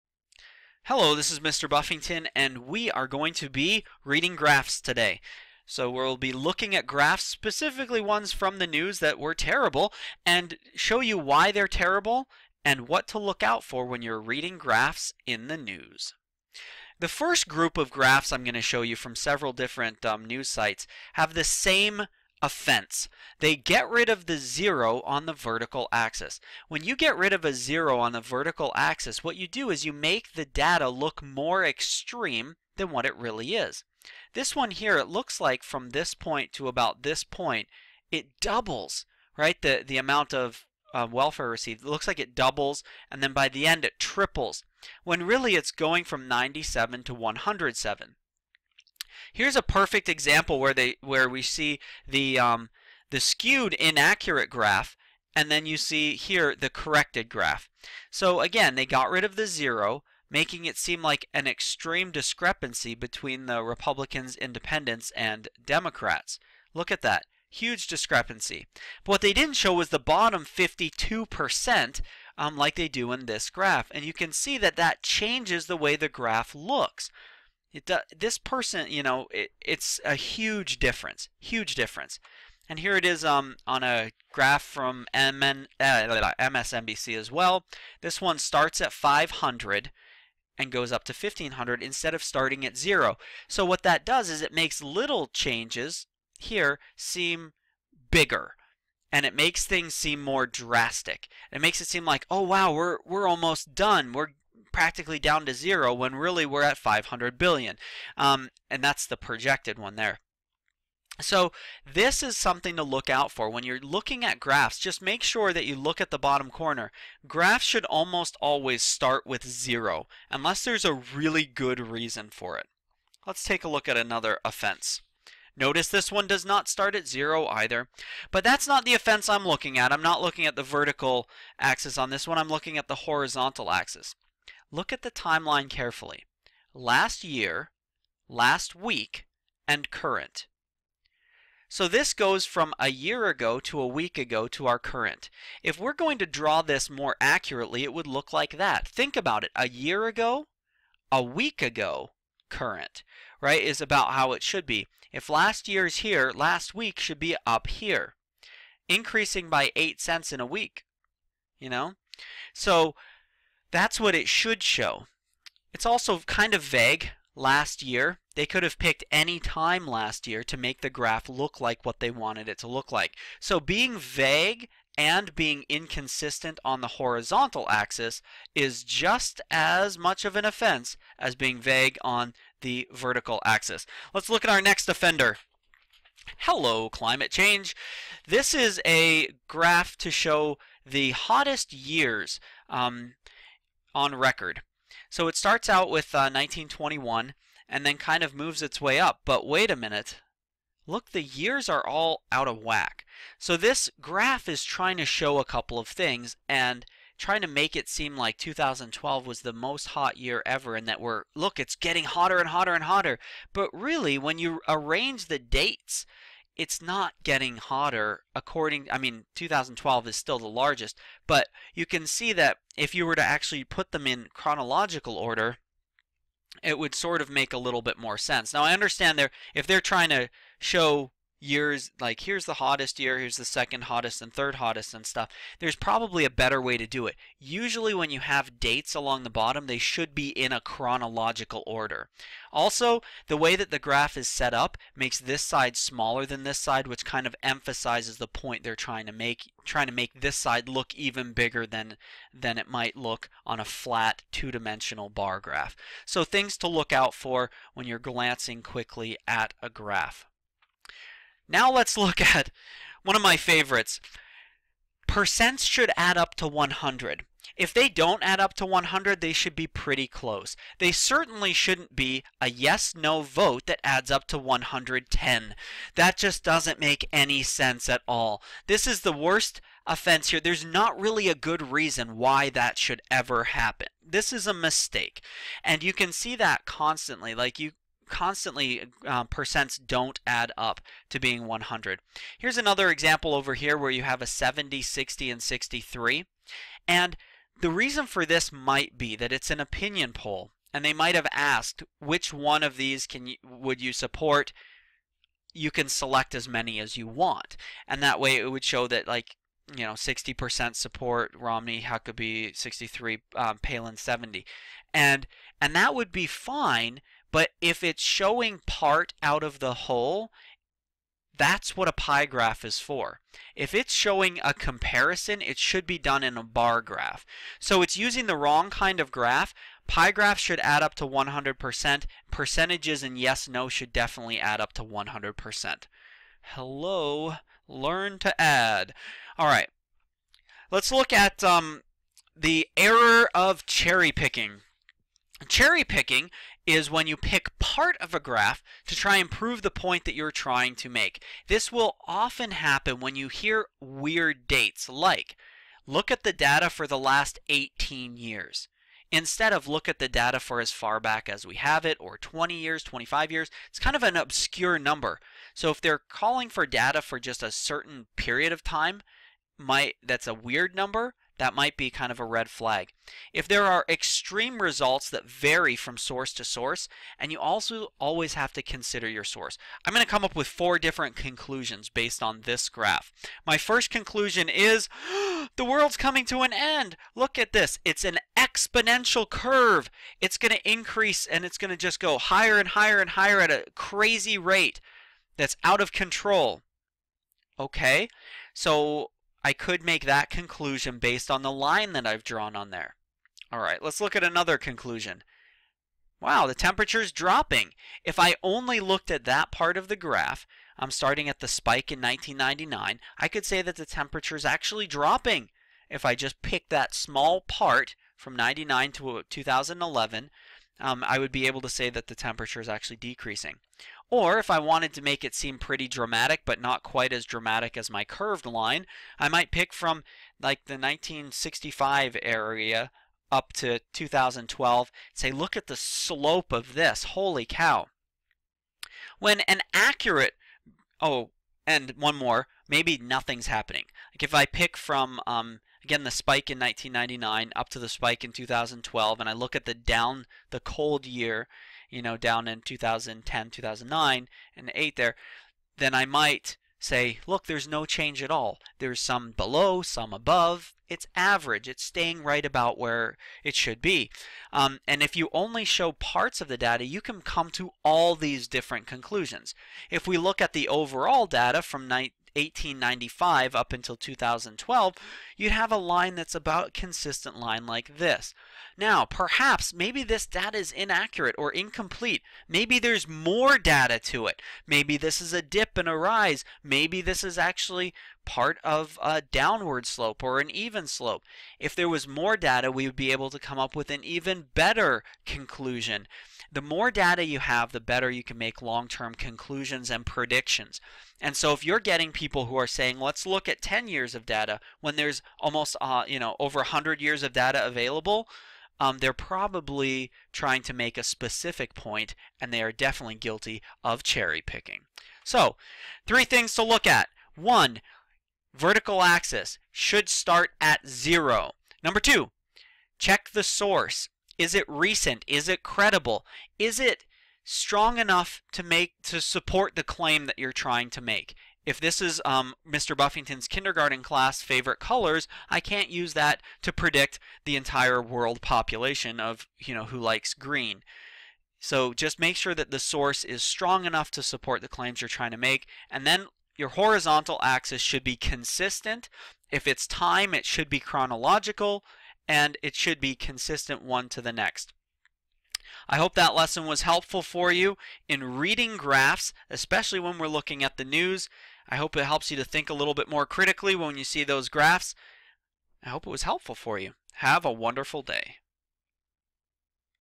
Fake News? How to Spot Misleading Graphs in the News!
In this clip you'll see common tactics news stations use to skew data in favor of the position they are promoting. By knowing these tactics you will be better able to critically read graphs on the news!